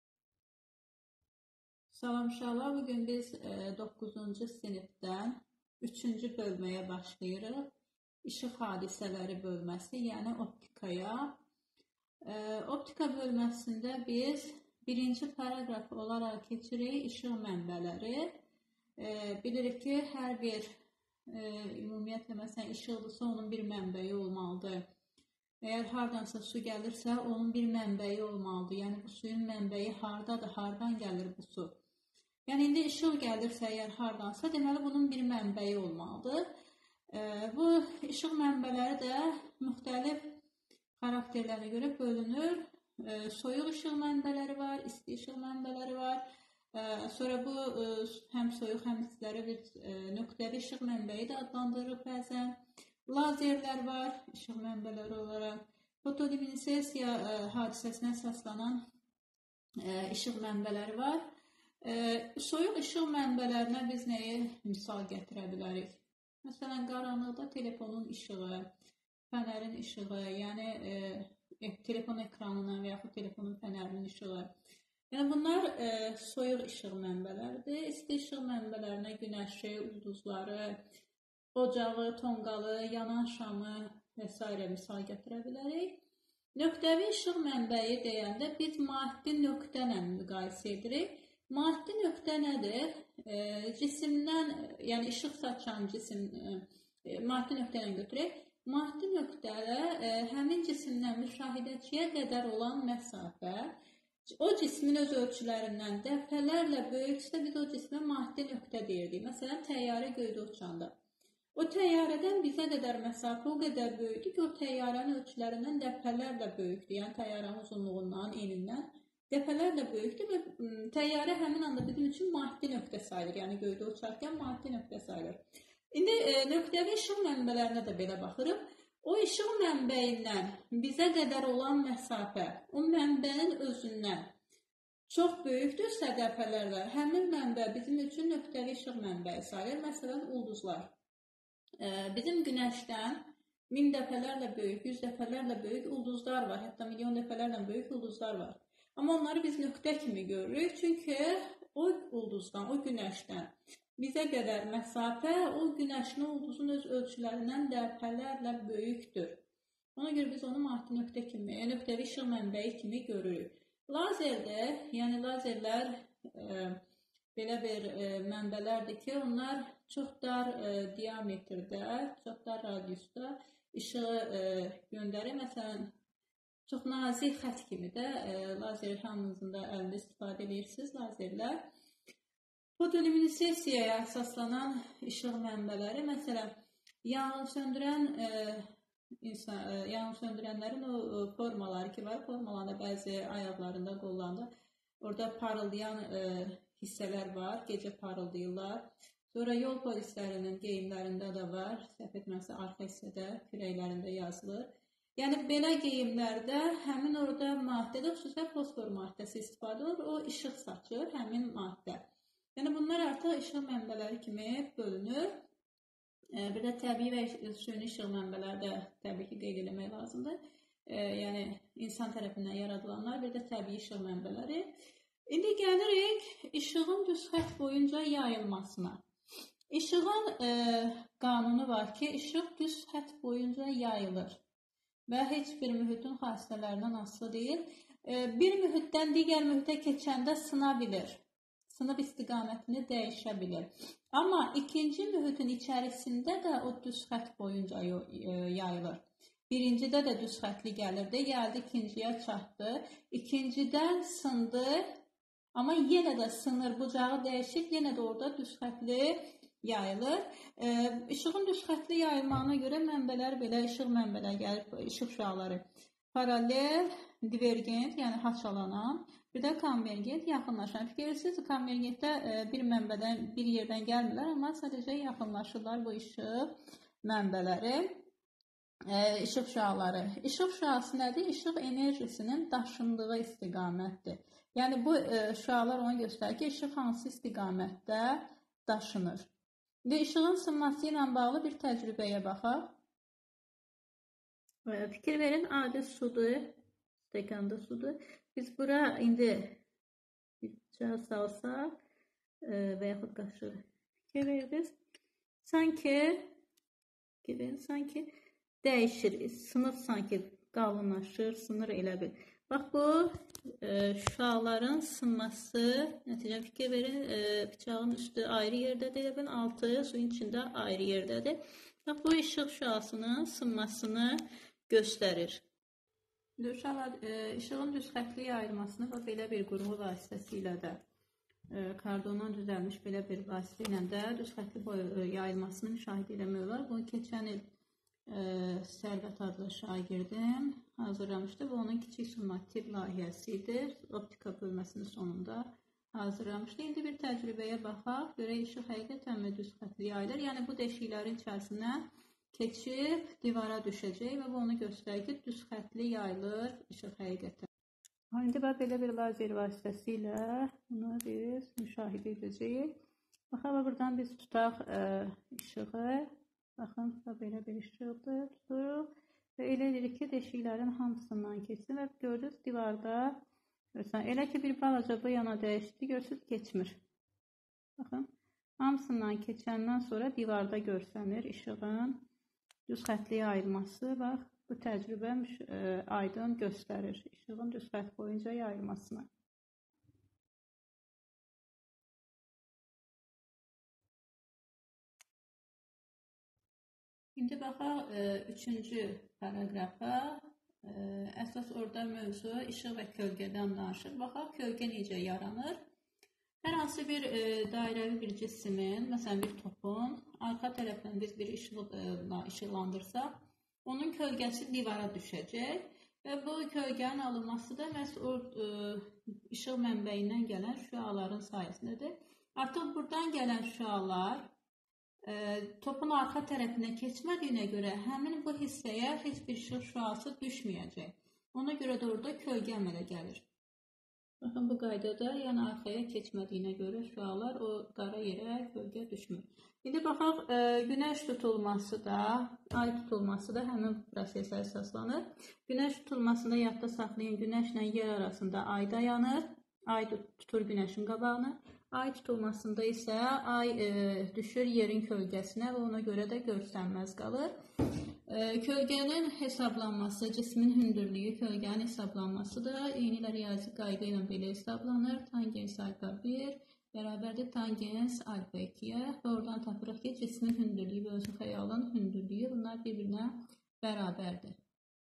(IX) 3.1.İşıq mənbələri. 3.2.İşığın düz xətt boyu yayılması. 3.3. İşıq və kölgə. Əhmədova Fidan
(IX) 3.1.İşıq mənbələri. 3.2.İşığın düz xətt boyu yayılması. 3.3. İşıq və kölgə. Əhmədova Fidan Şirinağa qızı. Siyəzən